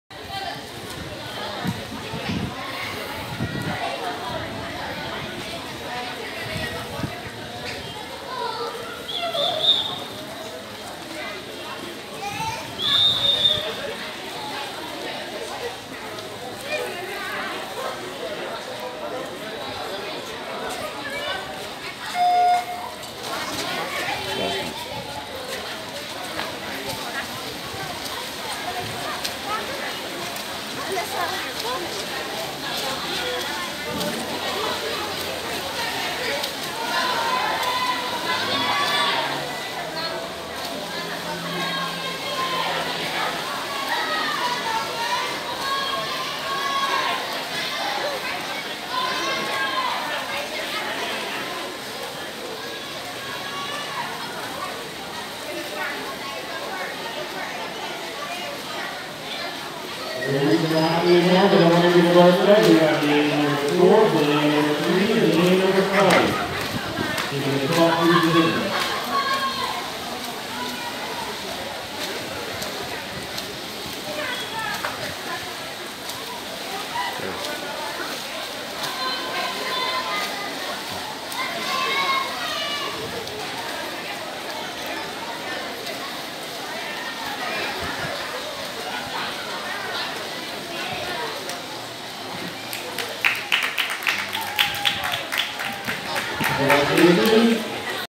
Tariq Greenidge 100m Fly BASA Short Course Nationals 2021 Dec 8th
100 Fly , Time :  1.09.27